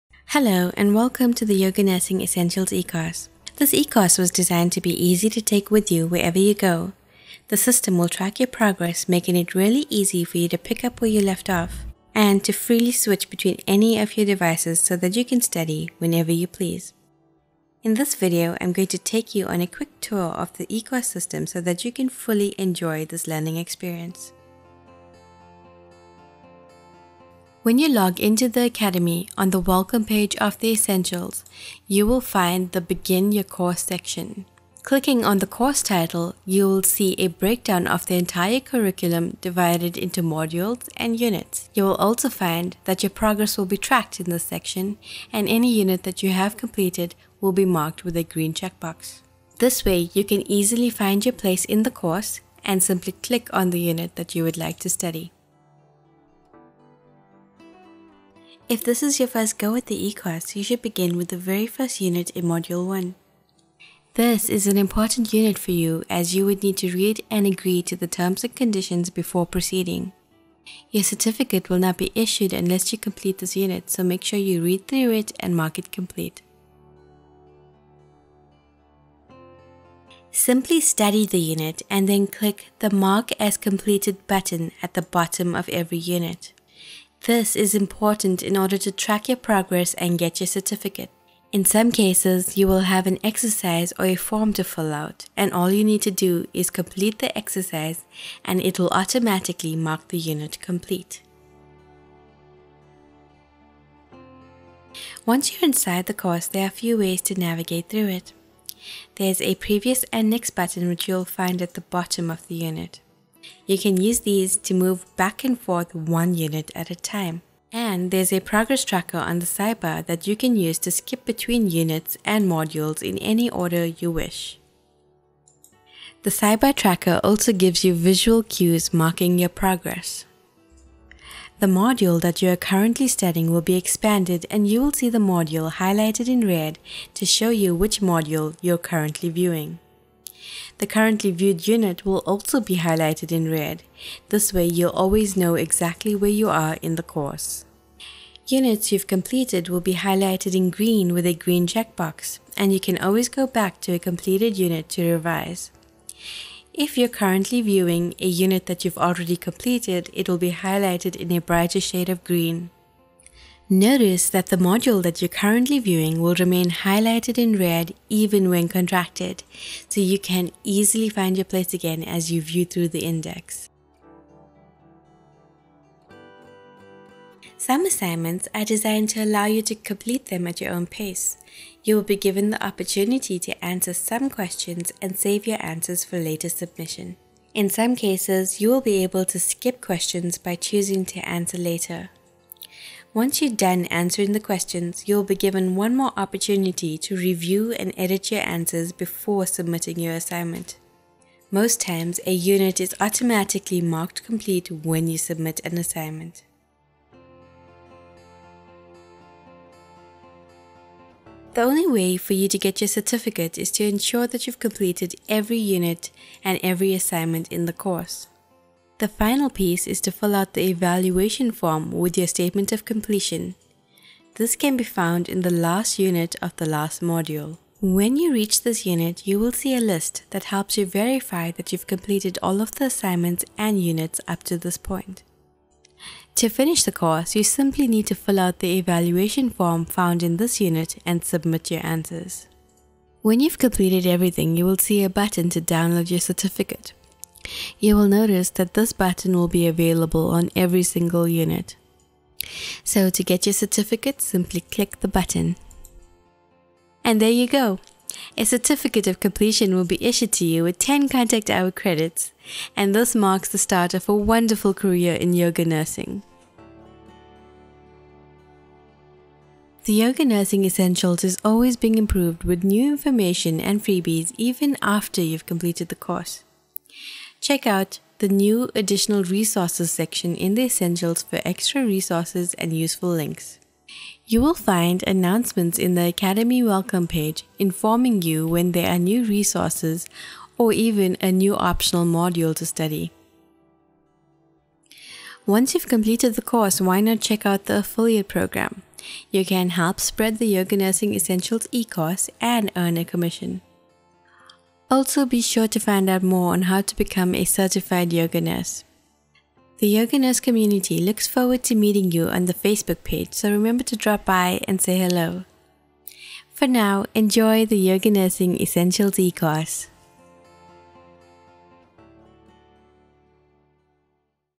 YN Academy eCourse Tutorial
Learn how easy it is to navigate inside the Yoga Nurse Academy eCourse. Members Get the YogaNursing Essentials,10 NCEs, certificate, research, community, free updates, access to new income streams and much, much more.